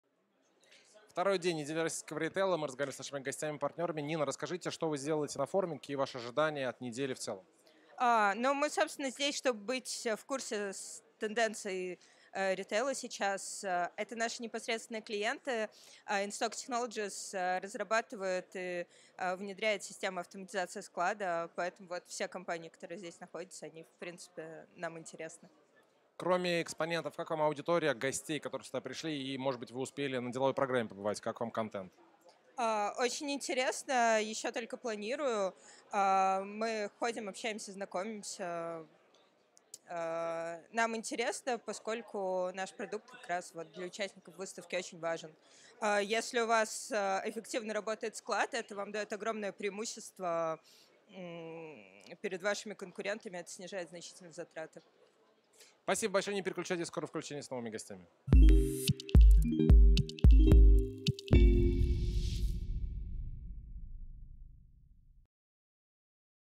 Нина Тухватулина, InStock Technologies
Нина Тухватулина, InStock Technologies, отметила, что компания находится на форуме, чтобы следить за тенденциями

Детали интервью в видео: